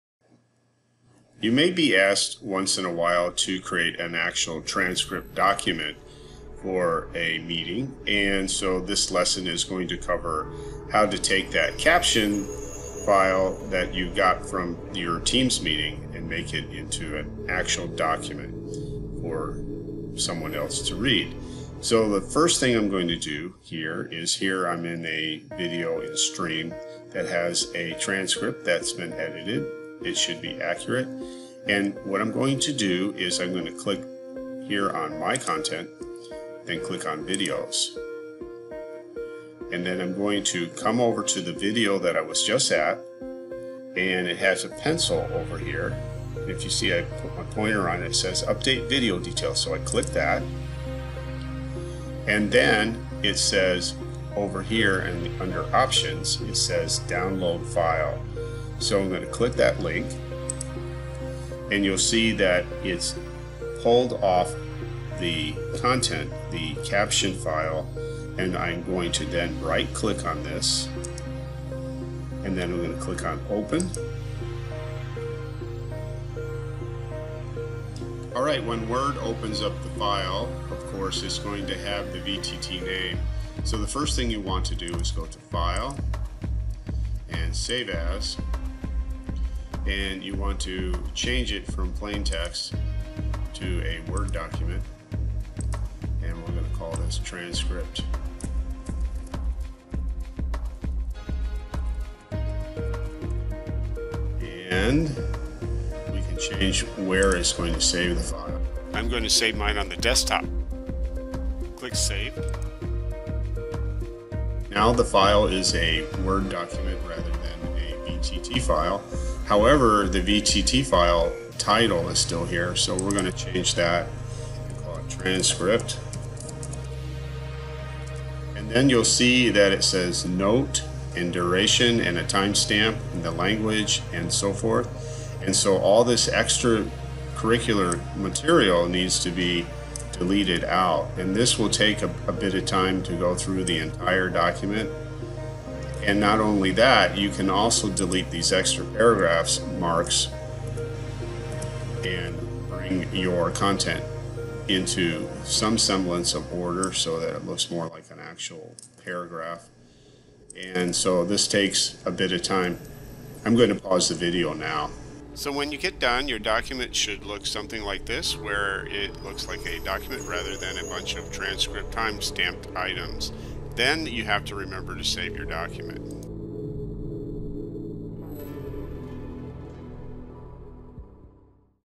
Making a transcript from the Teams Meeting Live Captions
This video covers how you can make a Word file from the live captions, so that  you can have a transcript to send to your class.  In order to do this you first must have been a host of a Teams Meeting and turned on the live captions, and then , of course, recorded that meeting.  The video ends up in the Stream video playform.  Tihs only covers how to extract that caption file into a Word file.